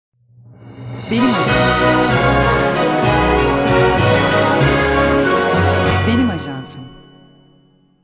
Hediye Paketi Temalı Yılbaşı Videosu (YLB015)
Bu yılbaşı sevdiklerinizin, müşterilerinizin ve takipçilerinizin yeni yılını Hediye Paketi Temalı Yılbaşı Videosu ile kutlayın.

Sizin için hazırladığımız bu yılbaşı video animasyonunda sizin logonuz veya isminizin yer almasını istiyorsanız satın alma işleminden sonra logonuzu (adınızı) ve varsa web adresinizi bize göndermeniz yeterli!

Benim Ajansım olarak 1 gün içerisinde size bu ayrıcalığı sunmaya hazırız.